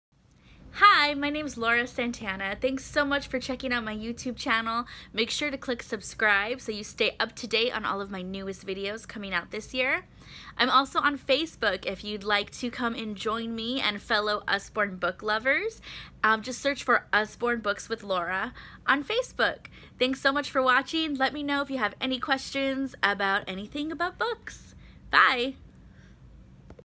Welcome to my YouTube Channel!
Thanks so much for visiting my YouTube Channel! My name is Laura Santana, and I live in Orlando, Florida. I LOVE books and reading. Please let me know if I can ever assist you in finding the perfect books for the little readers in your life!